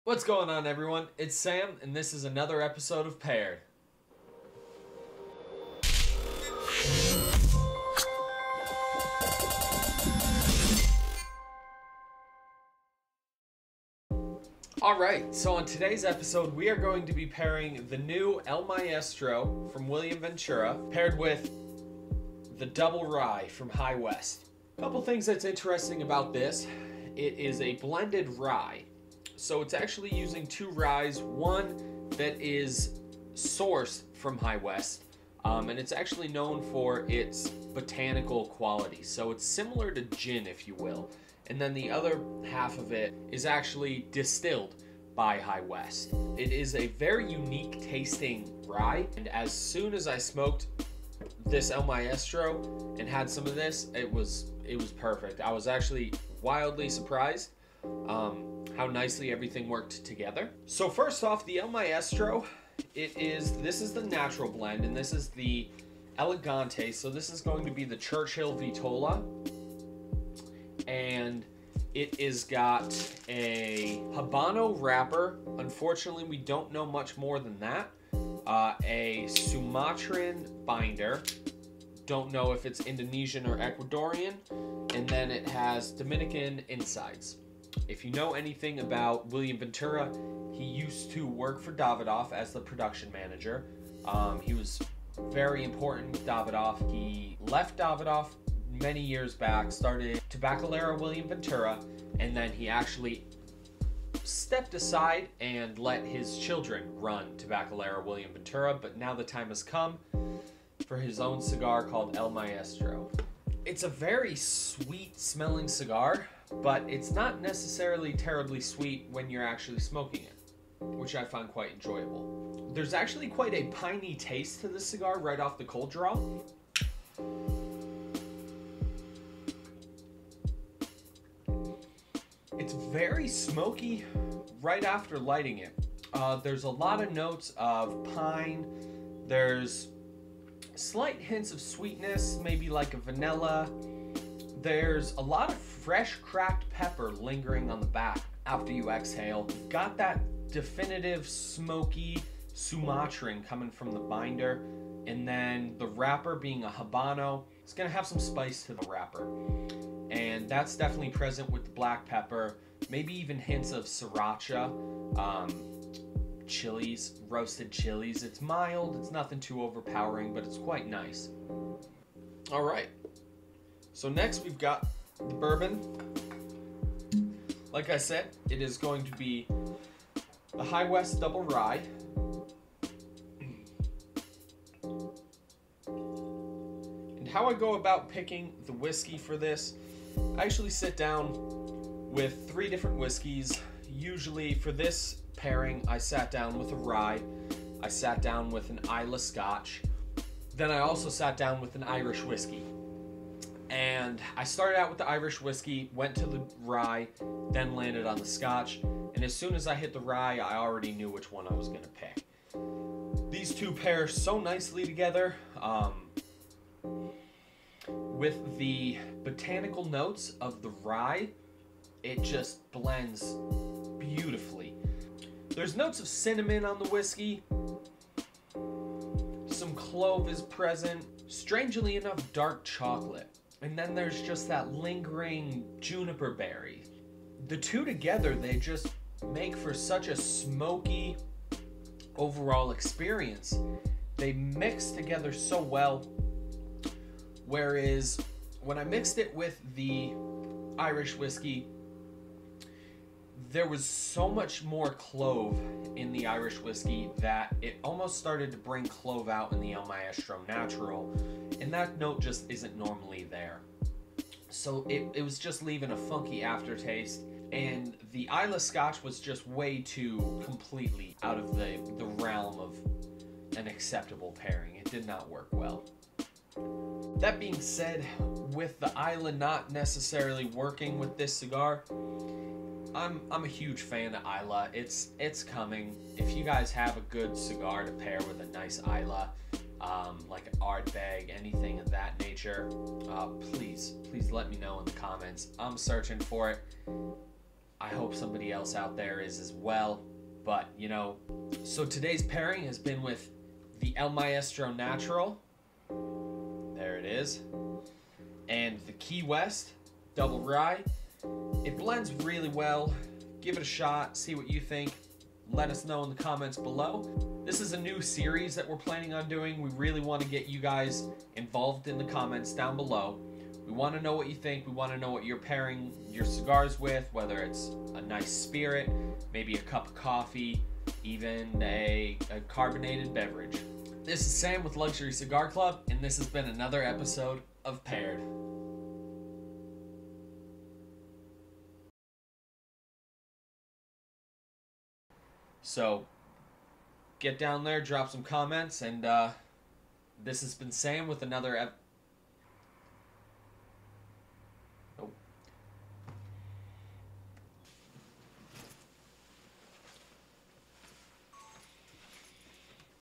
Paired E2 El Maestro
Paired Episode 2: El Maestro Natural Elegante

Cigar - El Maestro Natural Elegante

Churchill 7”x50

Wrapper: Habano

Binder: Sumatran

Filler: Dominican

Retail: $13.00

Whiskey - High West Double Rye!

Type: Blend of Straight Rye Whiskeys

ABV: 46%

Retail: $33.99

Welcome to Paired, where we pick a cigar, and a spirit or beverage to enjoy together. This episode highlights two rather easy-to-find pairings. The brand new El Maestro Natural Elegantes, and a blended rye whiskey. Let us know what you think of this pairing, and if you have found any excep-tional pairings yourself!